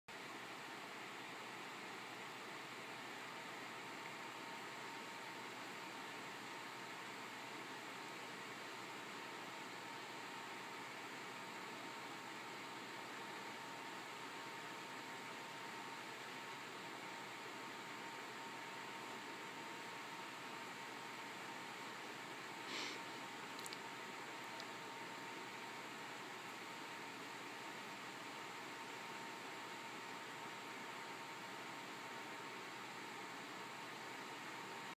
Kitty's version of Pooh and Hunny
Cat has discovered coconut oil. It's ok I researched it and it's good for her!